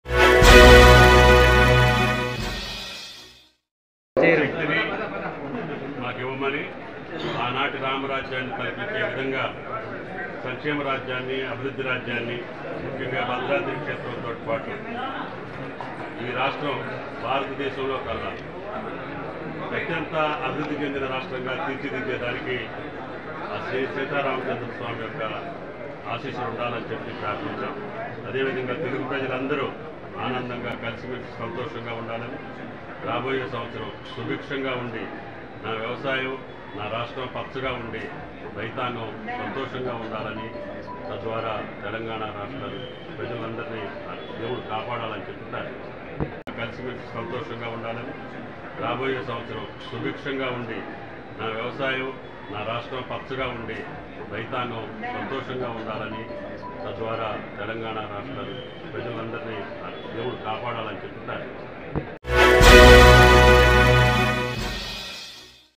దర్శించుకున్న...
తెలంగాణలో కాంగ్రెస్ పార్టీ అధికారంలో వచ్చిన తర్వాత  ముగ్గురు ఖమ్మం జిల్లా కీలక నాయకులు మంత్రి హోదాలో   ఖమ్మం జిల్లా భద్రాద్రి కొత్తగూడెం జిల్లాలలో పర్యటనలో భాగంగా  భద్రాచలం శ్రీ సీతారాముని ఆలయాన్ని దర్శించుకున్న   రాష్ట్ర ఉప ముఖ్యమంత్రి
శ్రీ మల్లు భట్టి విక్రమార్క గారు ,  మరియు
రాష్ట్ర వ్యవసాయ శాఖ మంత్రివర్యులు
*శ్రీ తుమ్మల నాగేశ్వరరావు గారు*,
 మరియు
రాష్ట్ర రెవెన్యూ శాఖ మంత్రివర్యులు
 శ్రీ పొంగులేటి శ్రీనివాస్ రెడ్డి గారు పాల్గొన్నారు
ఈ కార్యక్రమంలో తుమ్మల గారు మాట్లాడుతూ

రామరాజ్యం తలపించే విధంగా కాంగ్రెస్ పాలన
భద్రాద్రి శ్రీ సీతారామచంద్ర స్వామి వారి ఆశీస్సులతో వ్యవసాయం బావుండాలని కోరుకున్న
తెలుగు ప్రజలు సంతోషంగా ఉండాలి
వాస్తు ఆగమ నియమాలు పాటిస్తూ  మాడ వీధులు విస్తరణ చేపట్టాలని కలెక్టర్ కు సూచన
 ఈ కార్యక్రమంలో అశ్వరావుపేట ఎమ్మెల్యే ఆదినారాయణ గారు , పినపాక ఎమ్మెల్యే  పాయం వెంకటేశ్వర్లు గారు, సత్తుపల్లి ఎమ్మెల్యే మట్ట రాఘమయి గారు మాజీ ఎమ్మెల్సీ బాలసాని లక్ష్మీనారాయణ గారు, సాదు రమేష్ రెడ్డి గారు, రాయల నాగేశ్వరావు గారు,బ్రహ్మయ్య గారితో పాటు  జిల్లా కాంగ్రెస్ పార్టీ సీనియర్  నాయకులు పాల్గొన్నారు